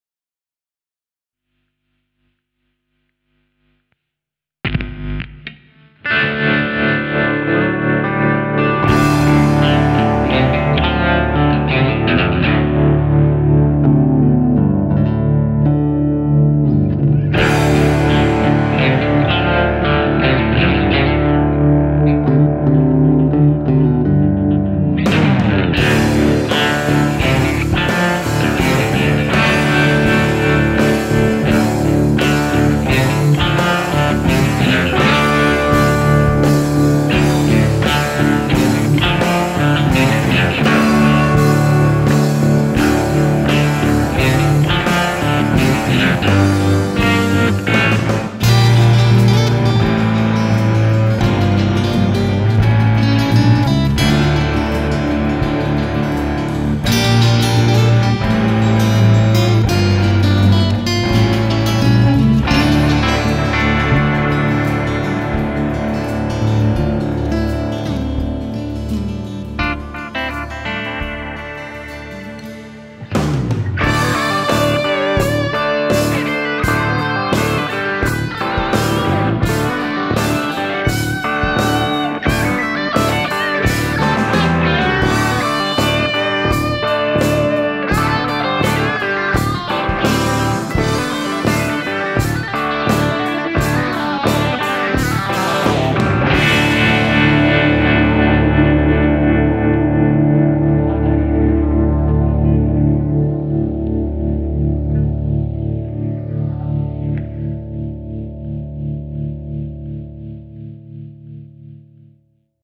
Introducing Blue Cat's Axiom V2: Limitless Guitar & Bass Software
We are thrilled to finally unleash the highly-anticipated Blue Cat's Axiom V2, a major leap forward in our guitar software, bridging the gap between beginners seeking simplicity and advanced users craving expanded capabilities.

All instruments by @guillaumejeulin.